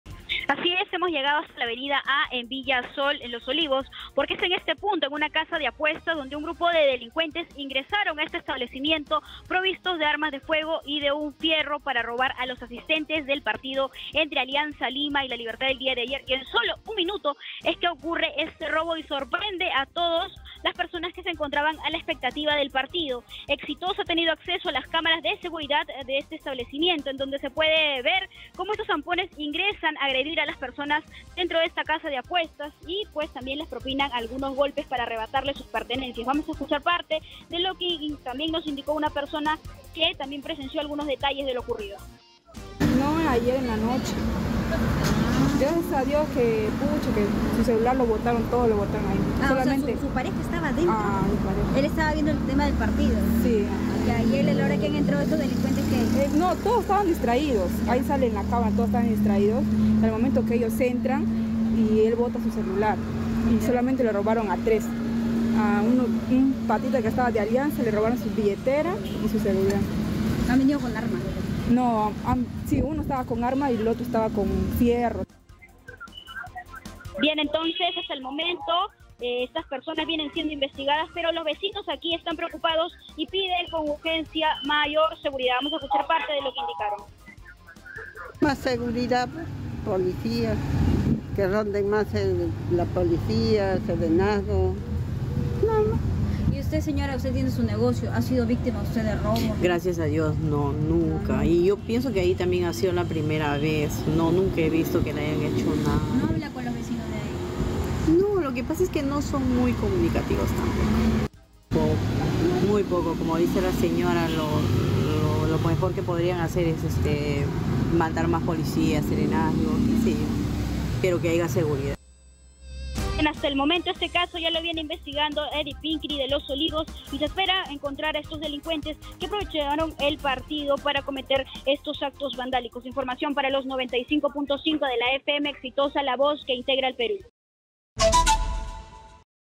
🔴🔵¡En pleno partido! Delincuentes asaltaron casa de apuestas en menos de un minuto
Los Olivos: Equipo de Exitosa tuvo acceso a las cámaras de seguridad de la casa de apuestas donde se observa a delincuentes que ingresan al establecimiento para robar a los asistentes en menos de un minuto.

Exitosa Perú
"Exitosa: La voz que integra al Perú"

Corporación Universal